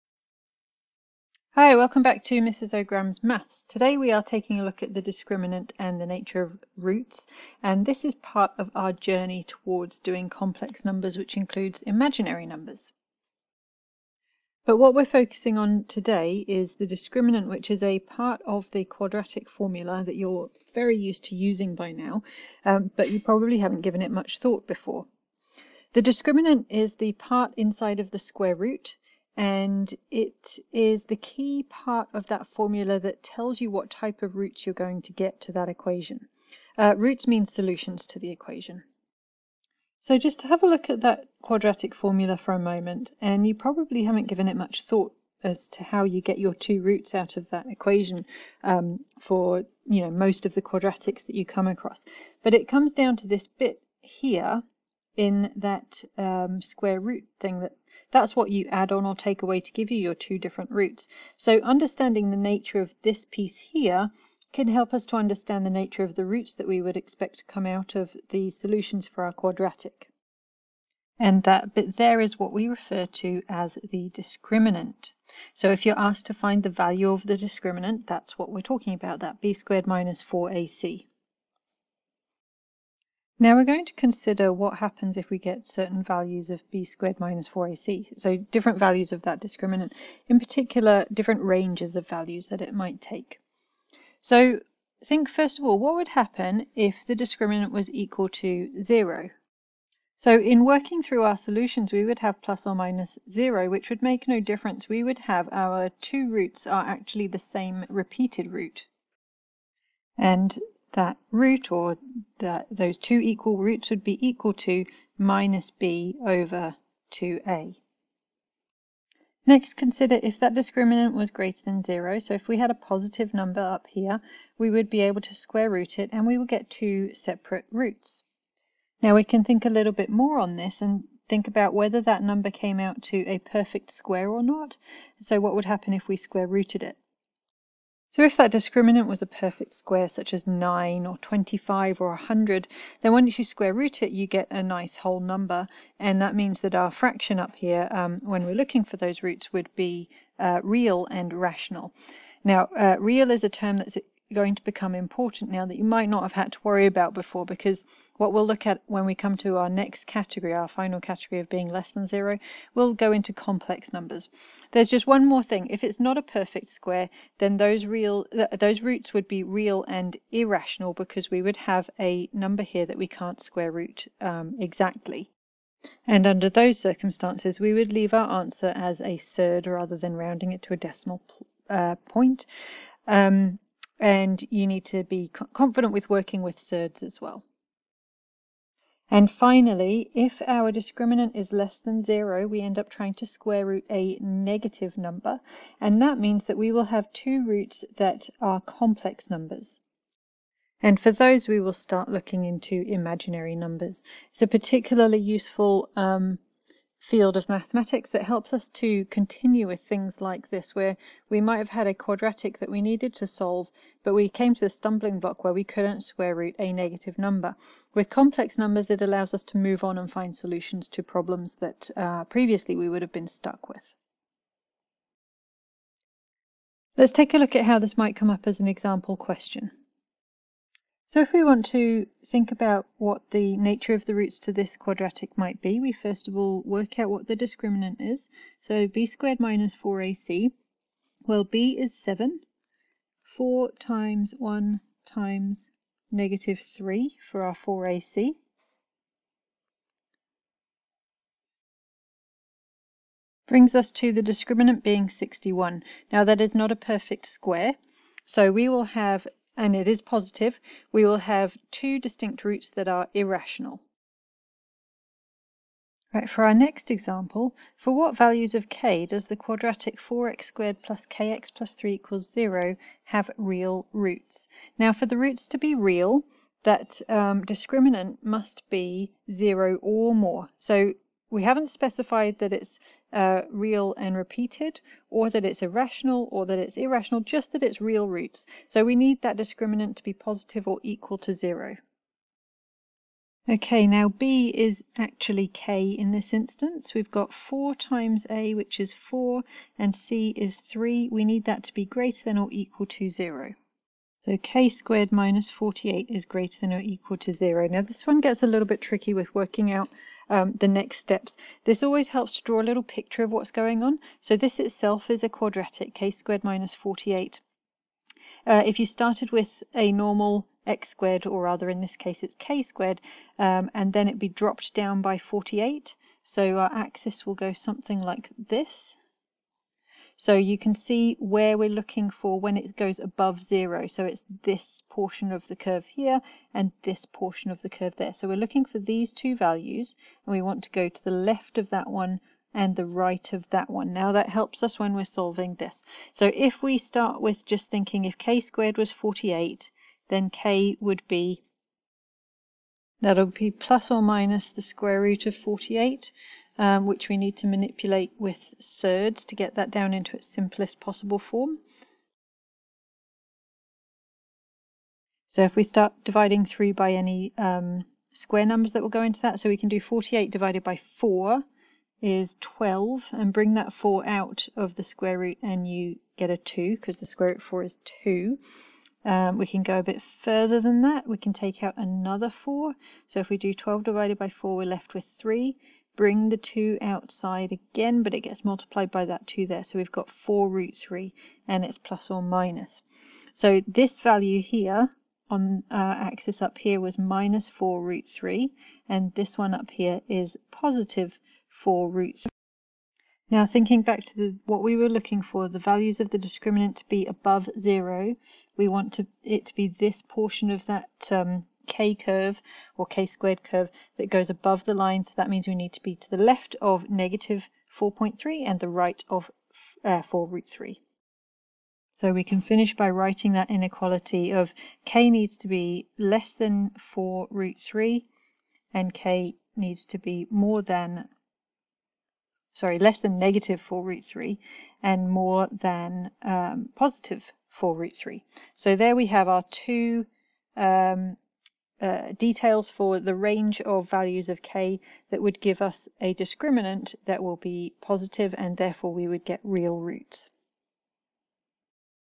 Discriminant and Nature of Roots
How to use the discriminant to determine the nature of quadratic roots (and why it works)